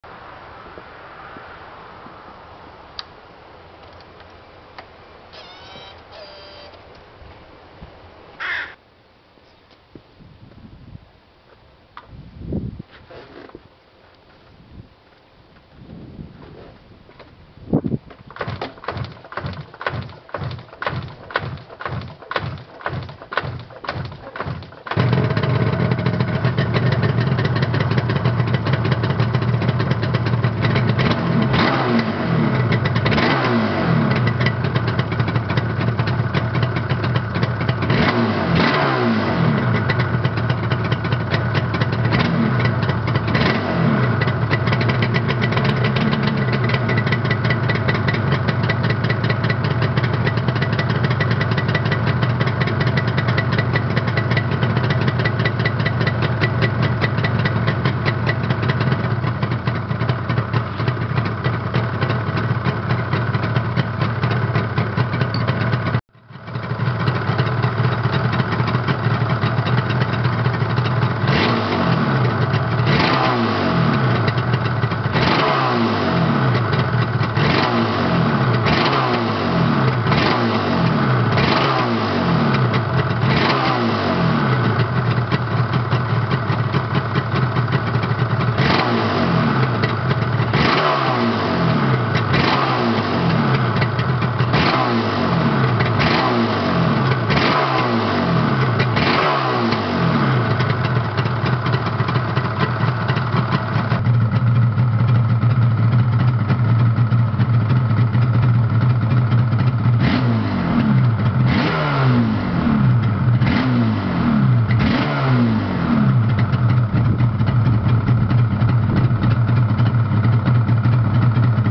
Yamaha RD350 YPVS (1990 RD350FII) cold start and revving.
Just a clip of my "new" RD350.Cold start with a little smoke. When the engine is warm there is no smoke at all.

Will try and capture a video when driving some time soon.